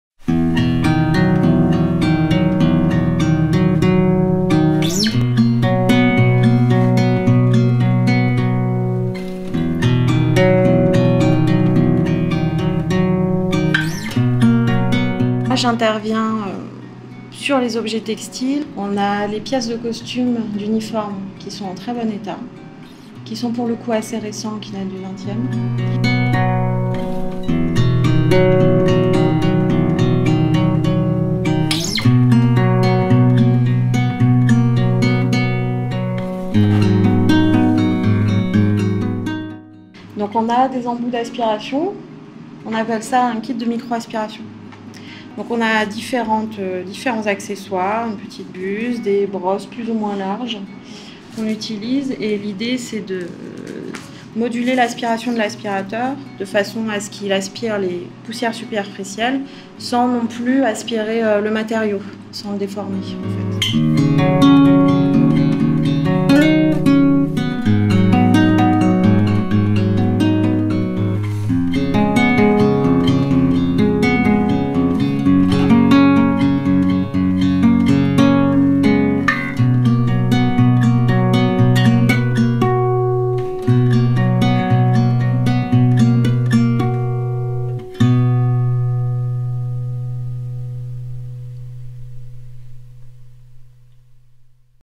Le traitement des tissus
Le Musée d'Armes, situé au Château de Joux, a fermé ses portes pour être entièrement rénové. Les collections ont du déménager temporairement au Musée Municipal de Pontarlier recevant, à cette occasion, des soins experts. Ces vidéos nous racontent ce transfert passionnant.

Retrouvez ici le zoom relatif au traitement des tissus.